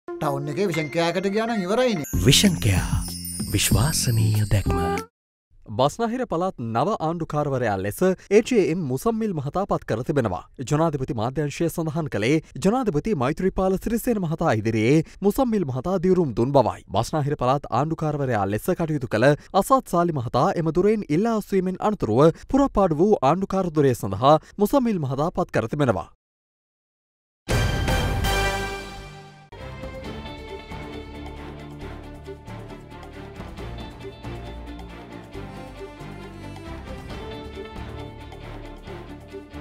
අසාද් සාලිගේ පුටුවට මුසම්මිල් එයි
Muzammil appointed Governor of Western Province